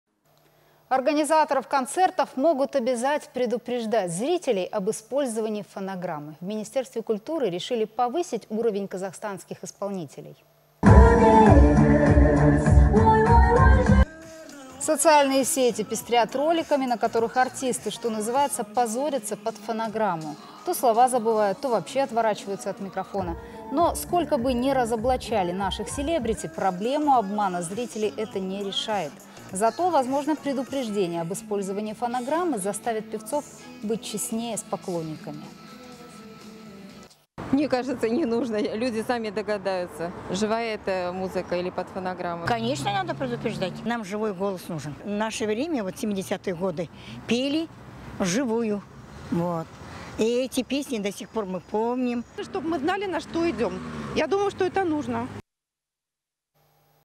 Организаторов концертов могут обязать предупреждать зрителей о фонограмме
Организаторов концертов могут обязать предупреждать зрителей об использовании фонограммы. В министерстве культуры решили повысить уровень казахстанских исполнителей.